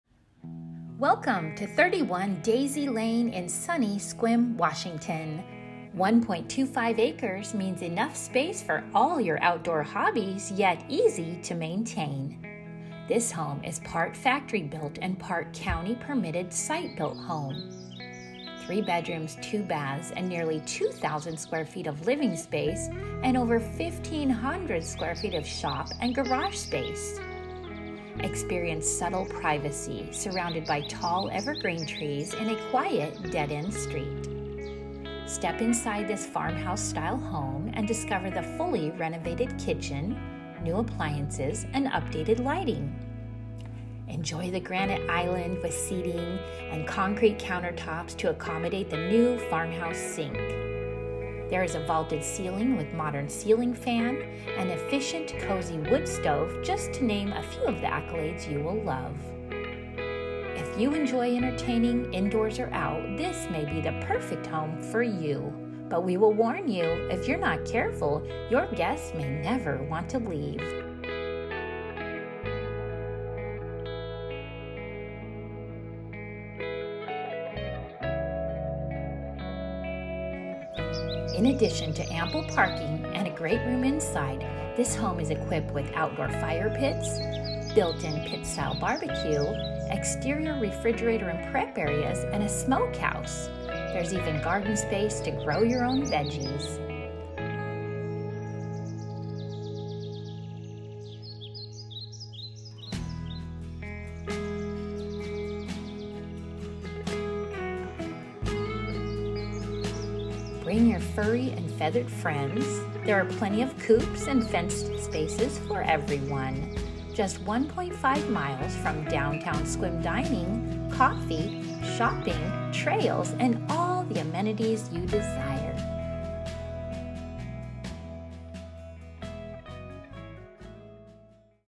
1.25ac Sequim Mini Farm With Charming Farmhouse In The Blue Hole
Welcome to 31 Daisy Ln, in Sunny Sequim Washington.
1.25 acres means enough space for all your outdoor hobbies yet easy to maintain.
This home is part factory built and part county permitted site-built home, 3 bedroom 2 baths and Nearly 2000sf of living space and Over 1500 sf of shop & garage space.
Experience subtle privacy, surrounded by tall evergreen trees in a quiet dead-end street.
Step inside this farmhouse style home and discover the fully renovated kitchen, new appliances, & updated lighting.
Enjoy the Granite Island with seating and concrete counter tops to accommodate the new farmhouse sink.
There is a vaulted ceiling with a modern fan and an efficient, cozy wood stove just to name a few of the accolades you will love.
If you enjoy entertaining, indoors, or out, this may be the perfect home for you! But we will warn you, If you are not careful your guest may never want to leave.
In addition to ample parking and a great room inside, this home is equipped with outdoor firepits, built in pit style BBQ, exterior refrigerator and prep areas and a smoke house and garden space!
Bring your furry and feathered friends, there are plenty of coops and fenced spaces for everyone.
Just 1.5 miles from downtown Sequim dining, coffee, shopping, the Olympic Discovery Trail and all the amenities you desire.